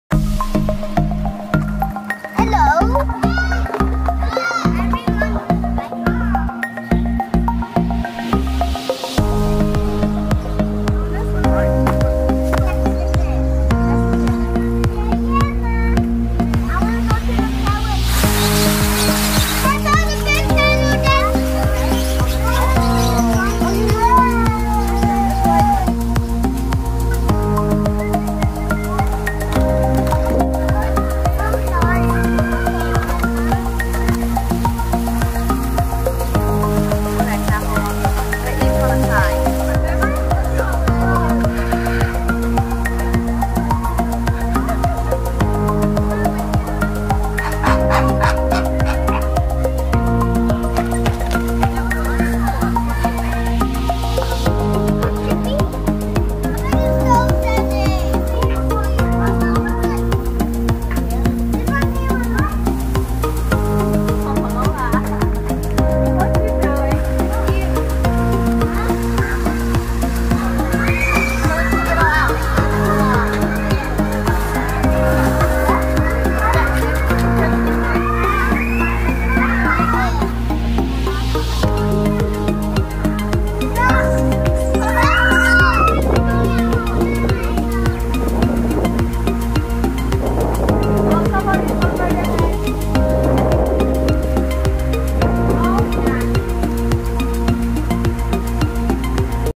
Auckland Zoo Feb 2020
Auckland Zoo. February 2020.
Bought a secondhand Sony Rx 10 IV to get the zoom I need to capture both the animals AND family's facial expressions.
Finding an Android Video Editor on my Huawei Mate 20 pro which allows me to edit the Sony footage was a bit of a mission.
Haven't read any instruction on the cam so far. Not a bad point and shoot video camera at all!